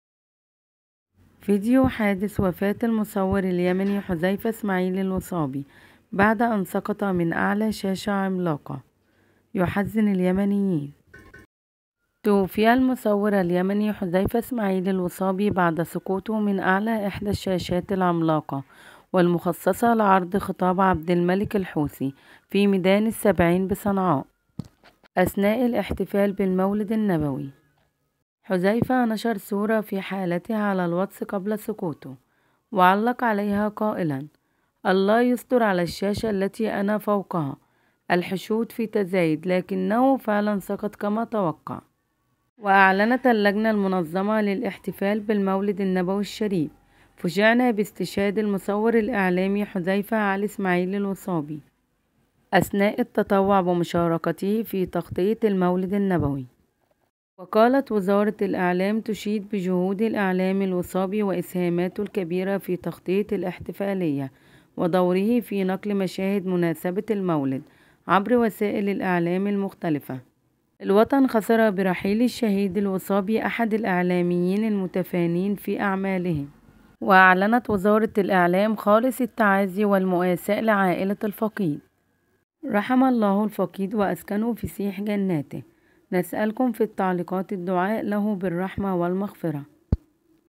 شاهدوا فيديو حادث المصور اليمني حذيفة اسماعيل الوصابي بعد سقوطه من أعلي شاشه عملاقه 😱 يحزن
تفاصيل وفاه المصور اليمني حذيفه اسماعيل الوصابي بعد سقوطه من فوق شاشة عملاقة في تغطية فعالية للحوثي حادث_وفاة_حذيفة_الوصابي_اليمن وفاة مصور يمني بعد سقوطه من فوق شاشة عملاقة أثناء تغطية فعالية للحوثيين اخبار محلية برس بي .. يمن مونيتور شاهد وفاة مصور يمني بعد سقوطه من فوق شاشة عملاقة أثناء تغطية فعالية للحوثيين والان مشاهدة التفاصيل. يمن مونيتور/قسم الأخبار توفي مصور تلفزيوني، مساء الأربعاء، إثر سقوطه من إحدى الشاشات العملاقة التي خصصت لعرض خطاب زعيم الحوثيين عبد الملك الحوثي. وقال شهود عيان إن المصور حذيفة إسماعيل الوصابي، سقط من أعلى إحدى الشاشات العملاقة والمخصصة لعرض خطاب عبد الملك الحوثي في ميدان السبعين بصنعاء. وأضافوا أنه جرى نقل الوصابي إلى أحد مستشفيات صنعاء، لكنه فارق الحياة بعد وصوله بدقائق. وتداول نشطاء حالة للوصابي على حسابه في واتساب، يقول فيها قبل سقوطه: “الله يستر على الشاشة التي أنا فوقها.. الحشود في تزايد، ليتحول ما كان يخاف منه إلى قصة محزنة أثارت ردود فعل واسعة بمواقع ______________________